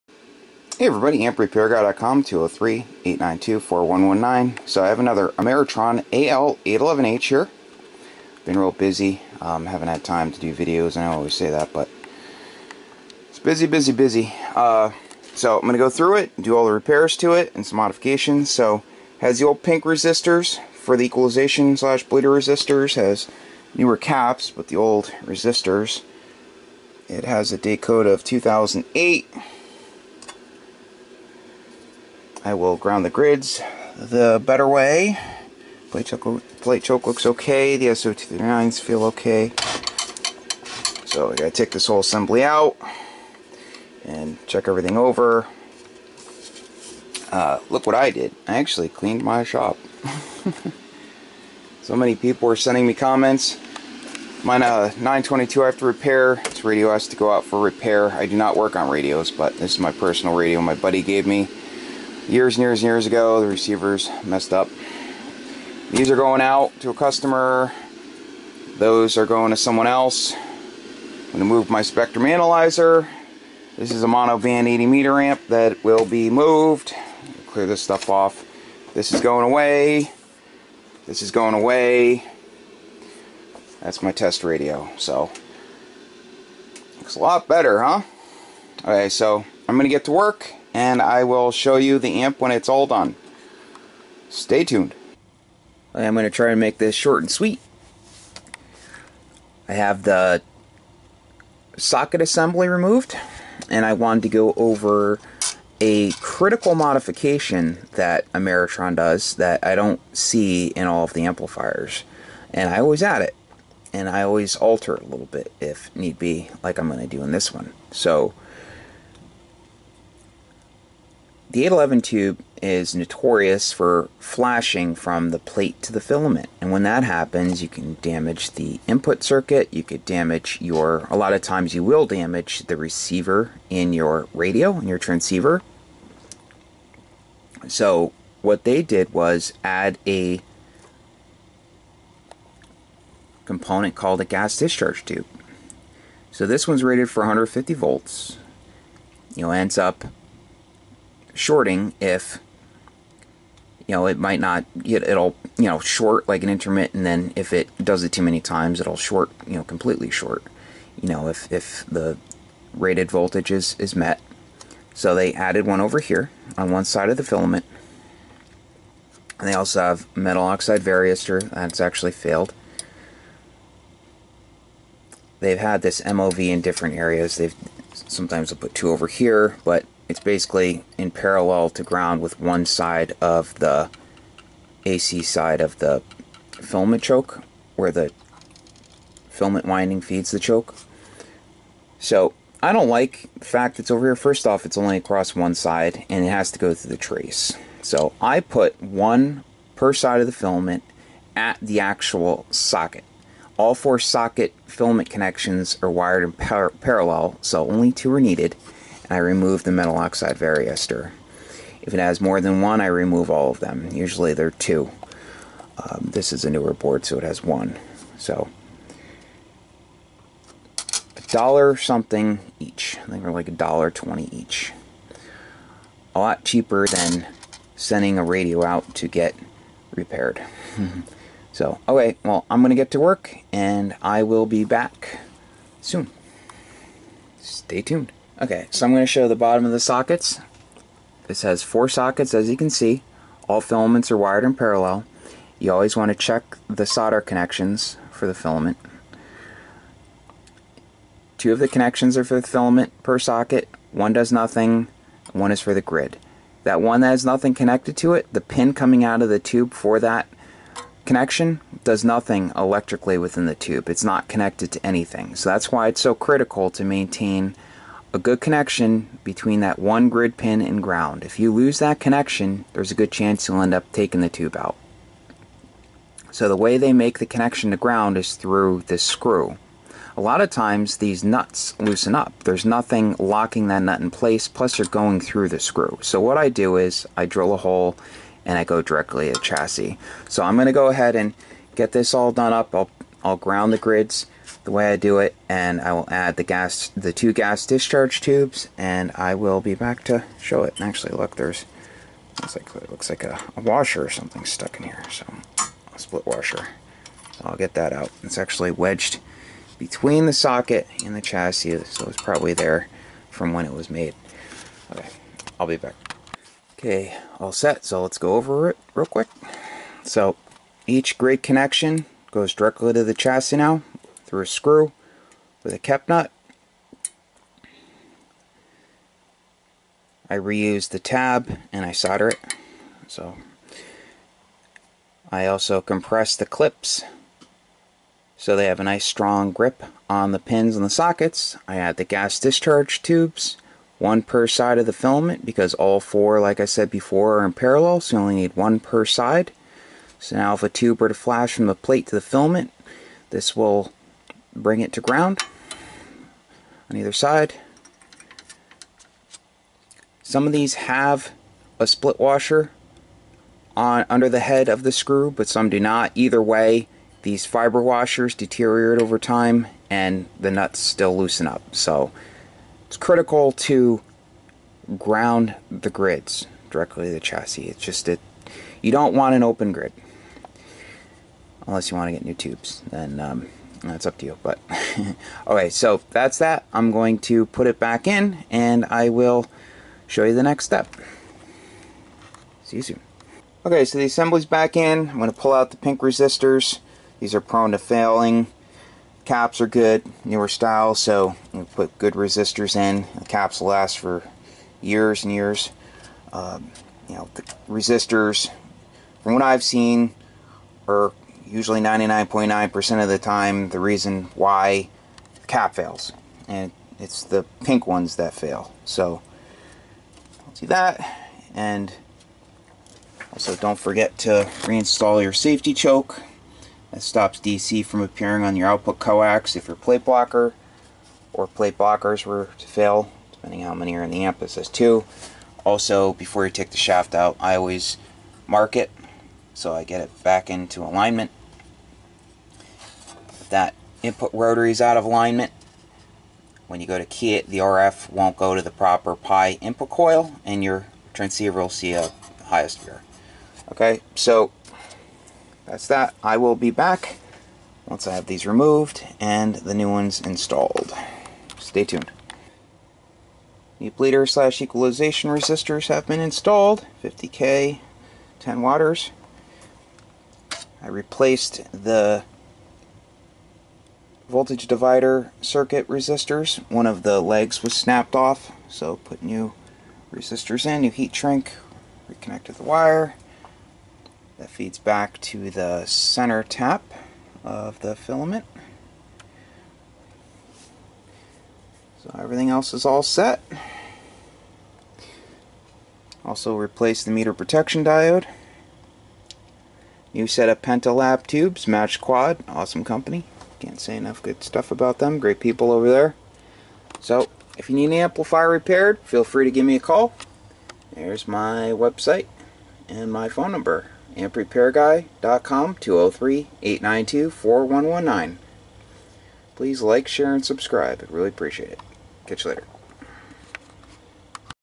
Ameritron Al811h Repair And Critical Modifications
Here I show the repair and modifications of a Ameritron al811h

Correction, Date Code is 20021 not 2008